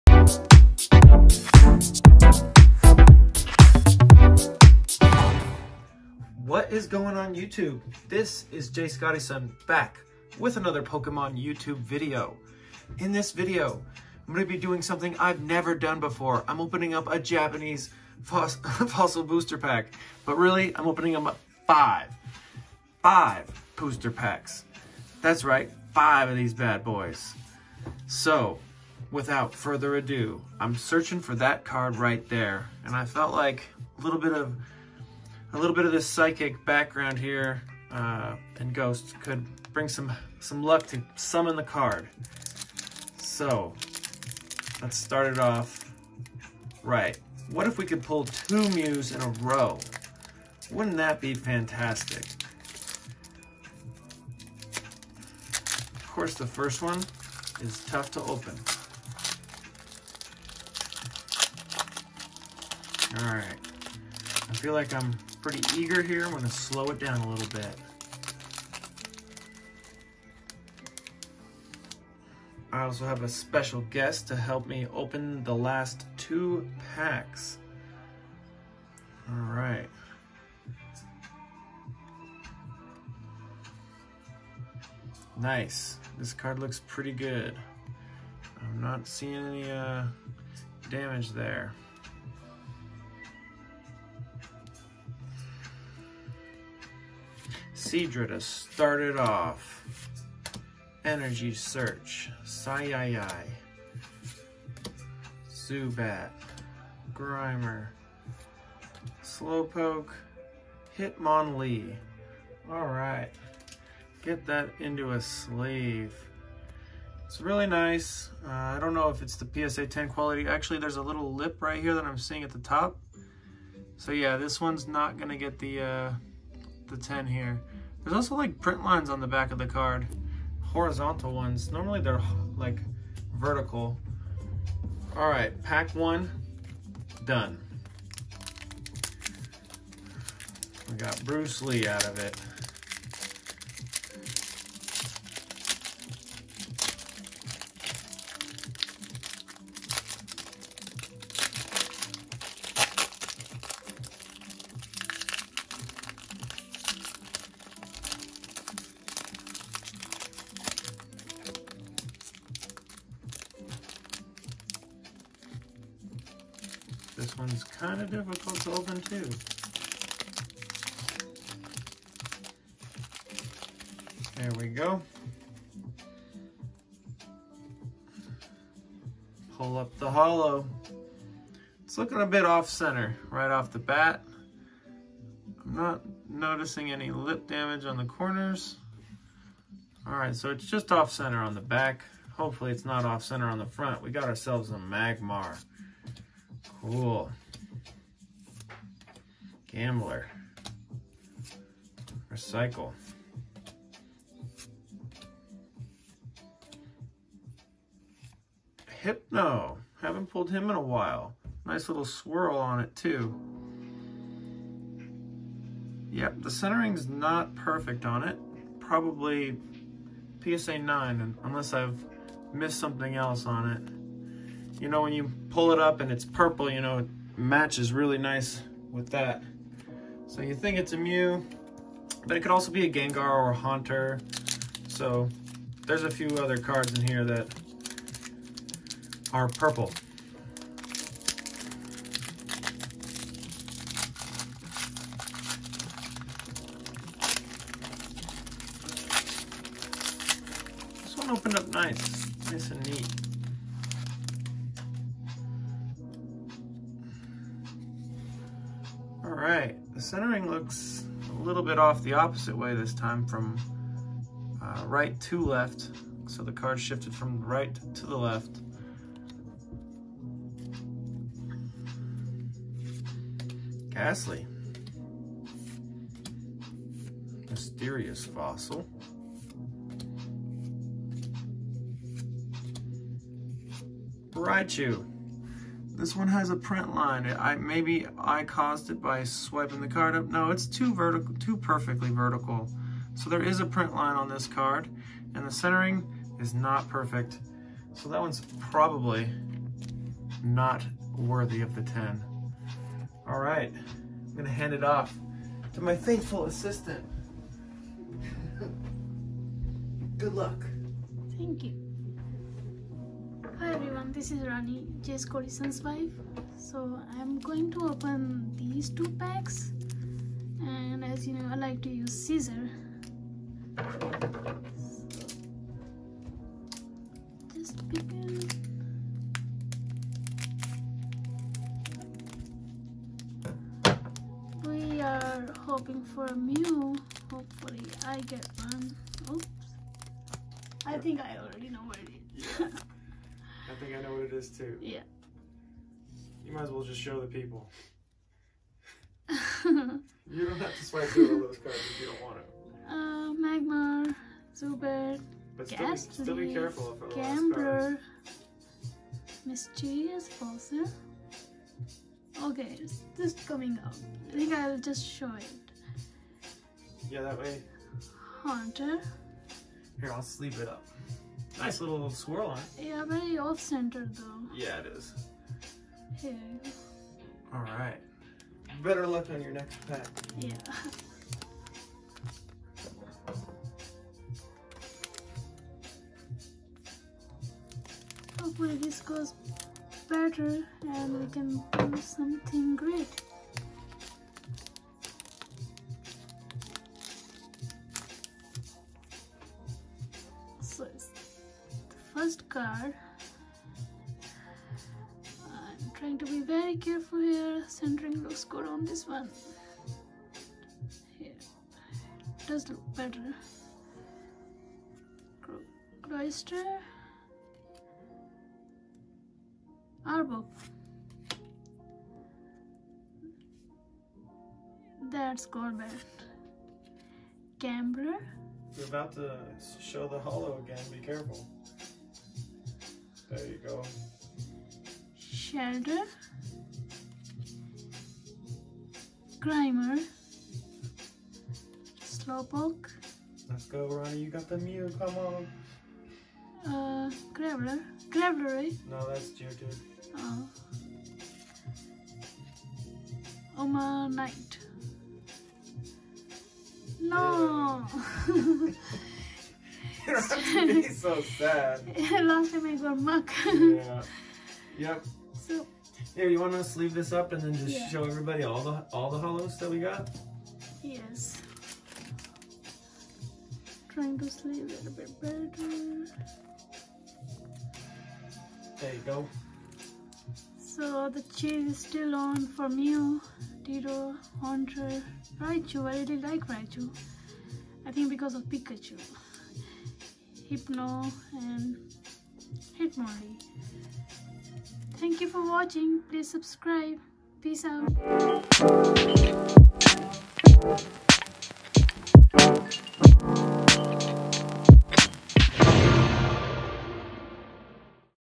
Opening up FIVE Japanese fossil booster packs!!! Search for Mew continues.
Instagram.com/i_grow_herb